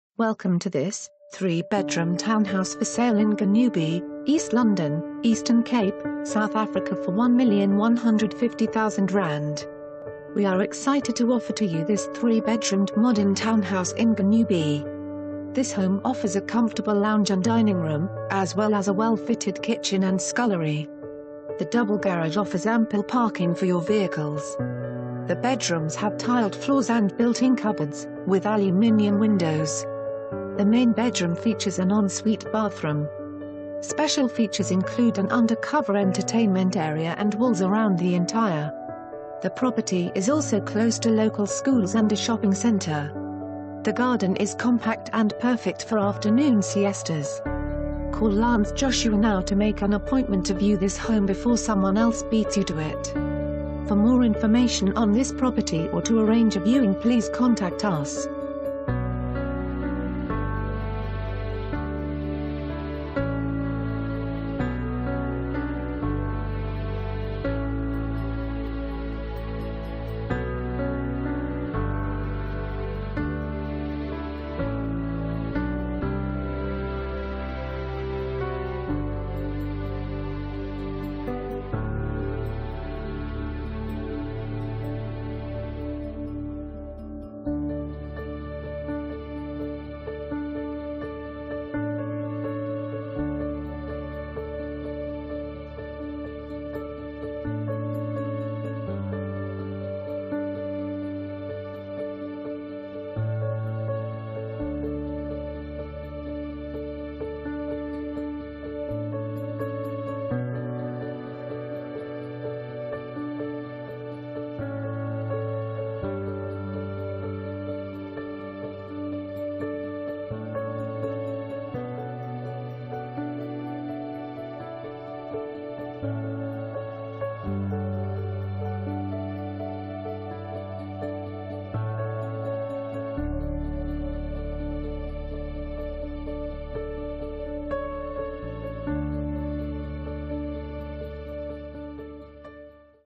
3 Bedroom Townhouse For Sale in Gonubie, East London, Eastern Cape, South Africa for ZAR 1,150,000
We are excited to offer to you this 3 bedroomed modern townhouse in Gonubie.

This home offers a comfortable lounge and dining room, as well as a well-fitted kitchen and scullery.  The double garage offers ample parking for your vehicles.

The bedrooms have tiled floors and built-in cupboards, with aluminium windows.  The mai...